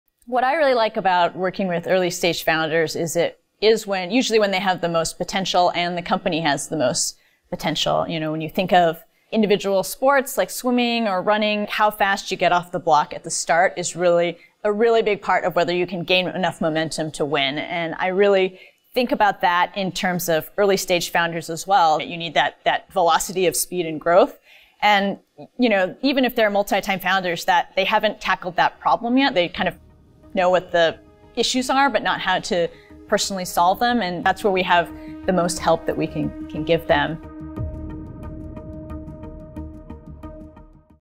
Why Jen Wolf Likes Working With Early Stage Founders
Initialized founder Garry Tan sits down with our new co-managing partners Jen Wolf and Brett Gibson to discuss the future of Initialized, what we look for in founders, and how we help our companies scale from 0 to 1.